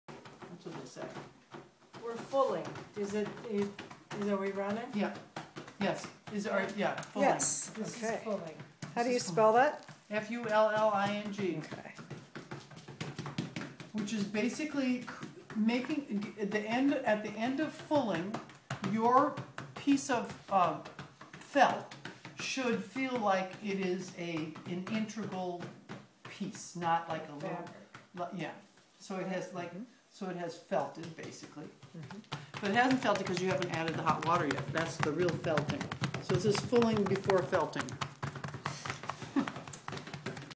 Fulling.mov
The "fulling" step during Kate and Sara's workshop on Nuno Felting.